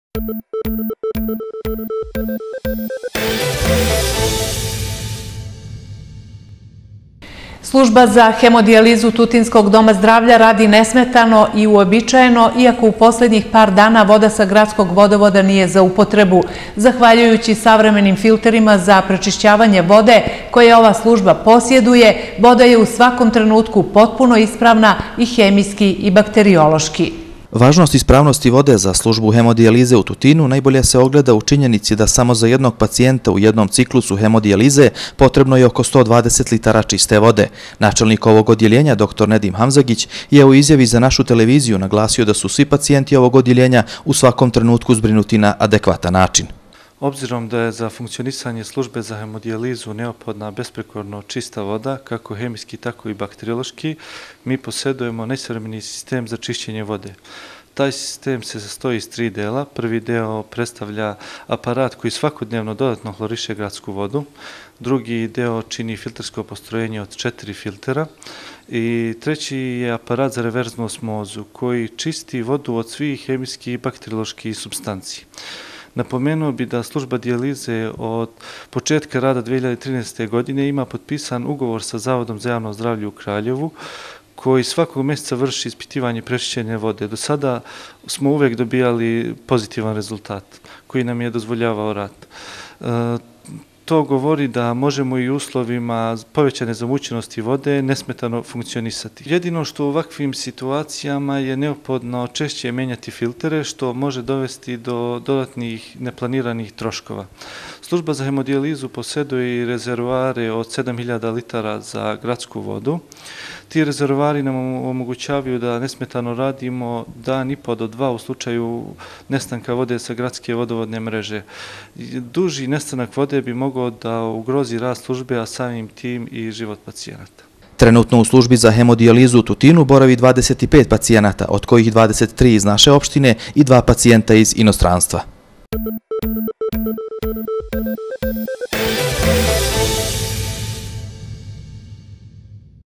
Dom zdravlja Tutin - povećana zamucenost gradske vode ne utiče na rad sluzbe za hemodijalizu
- Služba za hemodiajlizu u tutinskom Domu zdravlja radi nesmetano i uobičajno iako voda sa gradskog vodovoda nije za upotrebu, zahvaljujući savremenim filterima za pročišćavanje vode koja ova služba poseduje voda je u svakom trenutku potpuno ispravna i hemijski i bakteriološki.

- Vaznost ispravnosti vode za službu hedmoijalize najbolje se ogleda u činjenici da samo za jednog pacijenta u jednom ciklusu dijalize neophodno je 120 litara čiste vode. Načelnik odeljenja službe za hemodijalizu dr Nedim Hamzagić u izjavi za televiziju "Sandzacka tv mreza" je naglasio da su svi korisnici ovog odeljenja u svakom momentu zbrinuti na adekvatan način.

 "Obzirom da je za rad službe za hemodijalizu neophodna besprekorno čista voda kako hemijiski tako i bakteriološki, mi posedujemo najsavremeniji sistem za čišćenje vode iz gradskog vodovoda.

Sistem se sastoji iz tri dela,  aparat koji svakodnevno dodatno hloriše gradsku vodu, drugi deo je filtersko postrojenje od 4 filtera,  treći deo je  aparat za reverznu osmozu, koja čisti vodu od svih bakterioloških i hemijskih supstanci.

Od početka rada sluzbe potpisan je ugovor sa Zavodom za javno zdravlje u Kraljevu koji ispituju svakog meseca kvalitet prečišćene vode. Tako da i u danima povecane zamucenosti vode mozemo nesmetano fukcionisati.

Jedino što u ovakvim situacijama je neophodno menjati filtere znatno češće što može dovesti do dodatnih neplaniranih troškova.

Napomenuo bih da služba dijalize poseduje i rezervoare za vodu od 7 hiljada litara, tako da mozemo nesmetano funkcionisati  oko dan ipo do dva dana. Jedino nestanak vode duži od tog perioda može ugroziti rad službe a samim tim i život pacijenata".